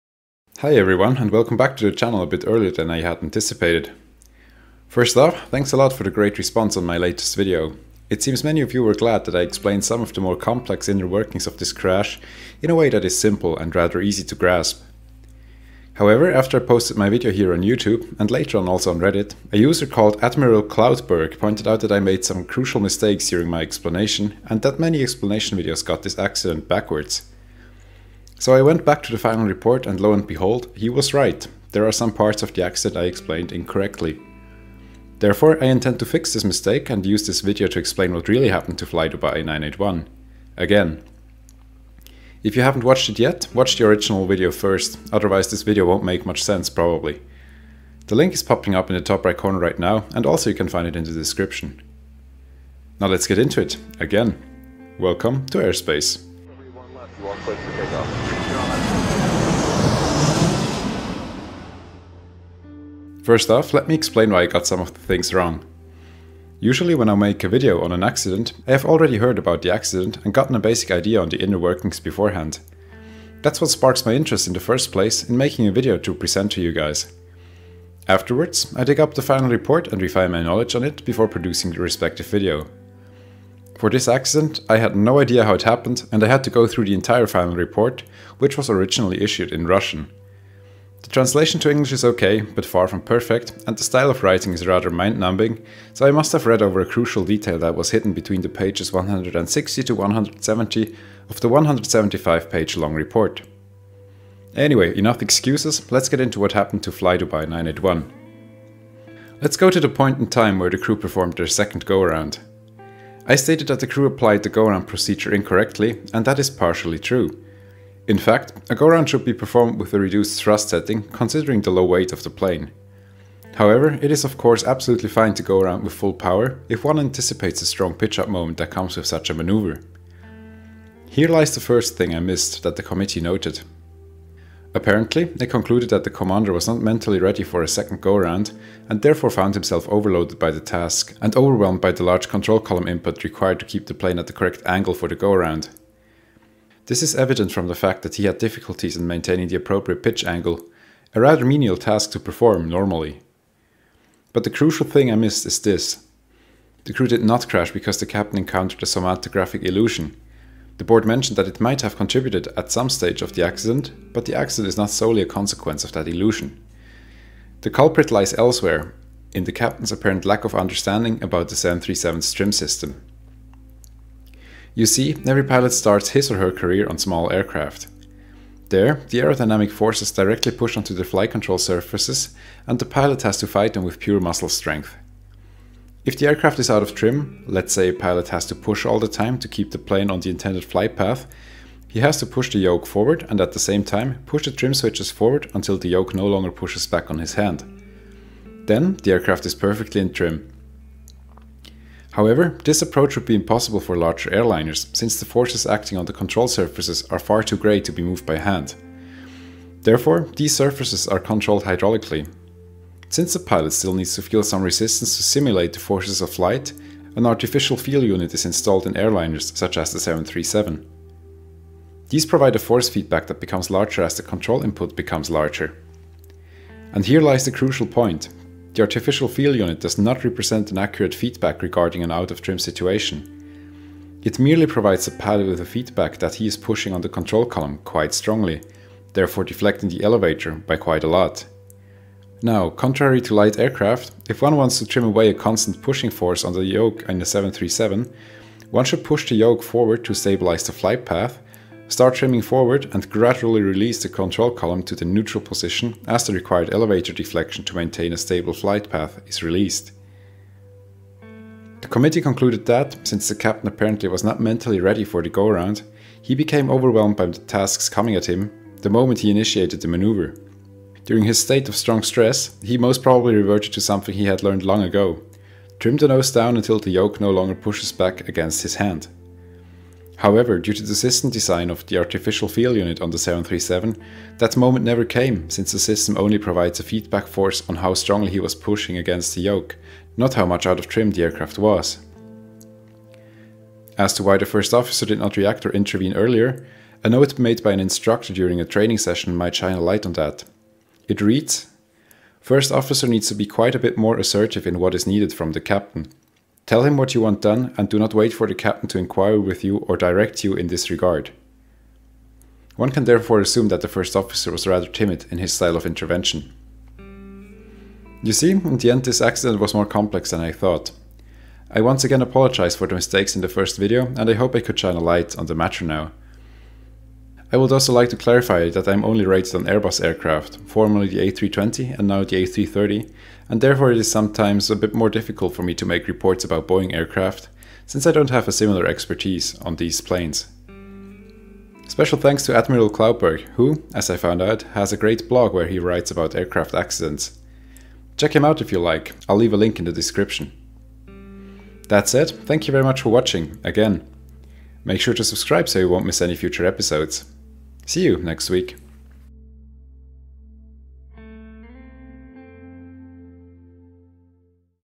Correction to Flydubai 981 | Disoriented Captain
Flydubai 981 was on approach to Rostov-on-Don, Russia, when unfavourable weather conditions forced the crew of the Boeing 737 to go around. But just moments later, the aircraft crashed onto the Runway. What happened to Flydubai 981? Find out what the pilots of the Boeing 737 did and how a strange illusion caused this accident.

Chapters
Story - 0:31
Trim Systems - 2:47

I am an airline pilot bringing you accurate and undestandable reports on past aviation disasters, plane crashes and weekly news in aviation.